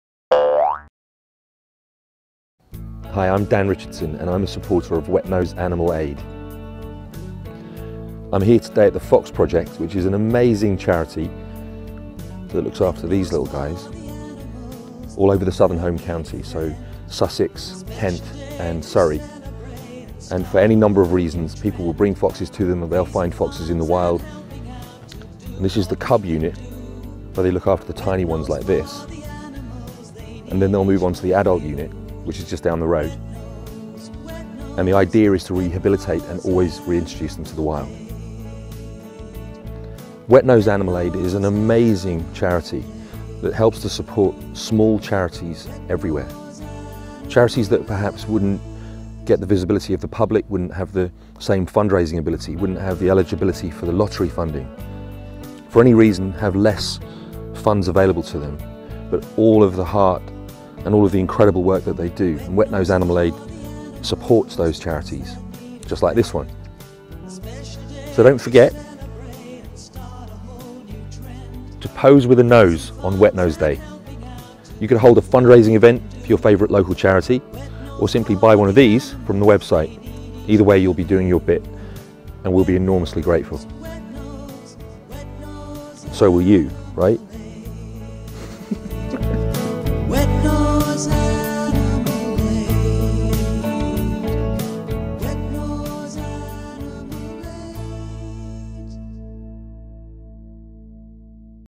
Wetnose Day film clip by Dan Richardson filming at the Fox Project
Wetnose Day film clip by actor Dan Richardson filming at the Fox Project Centre helping to promote our fundraising day